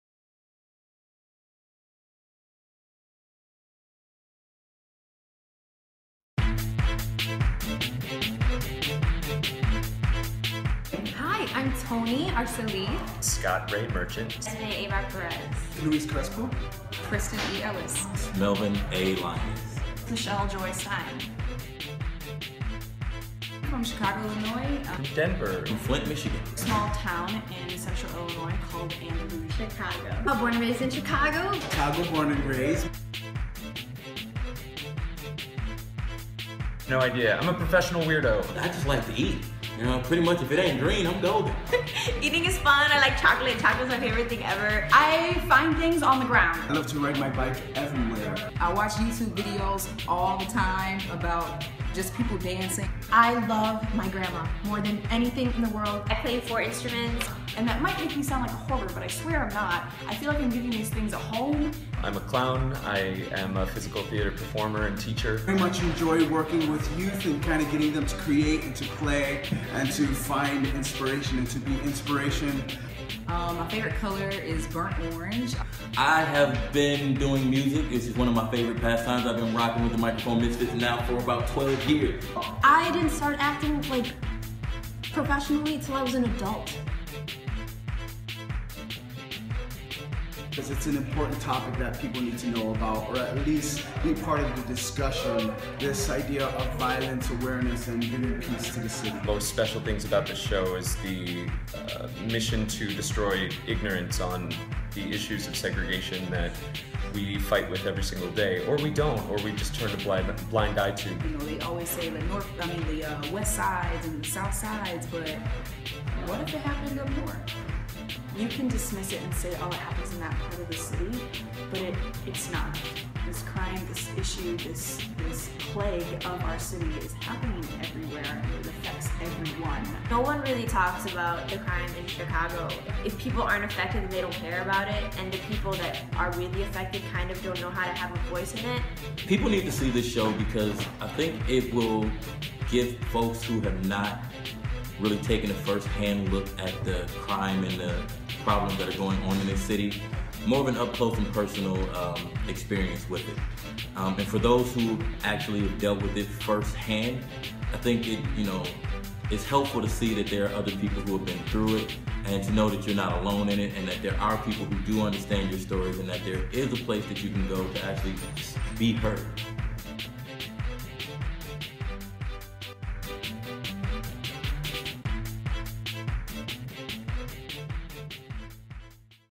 Meet the Cast of Crime Scene: The Next Chapter
Meet the cast of Crime Scene: The Next Chapter, the world premiere sequel to Crime Scene: A Chicago Anthology.

The Next Chapter starts previews May 21 in the Pentagon Theatre at Collaboraction and runs until June 21.